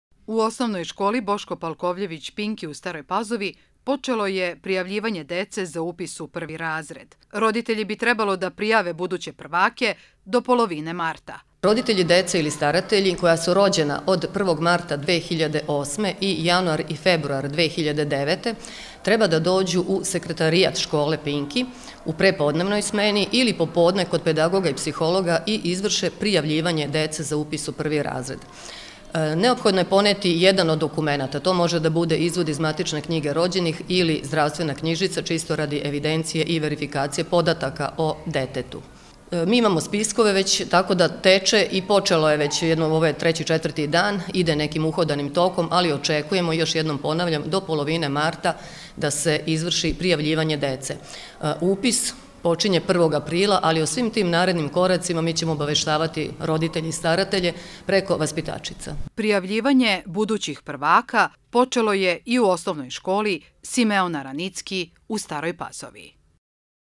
Prijavljivanje prvaka
U osnovnim školama počelo je prijavljivanje  budućih prvaka.Upis dece u prvi razred počinje 1. aprila.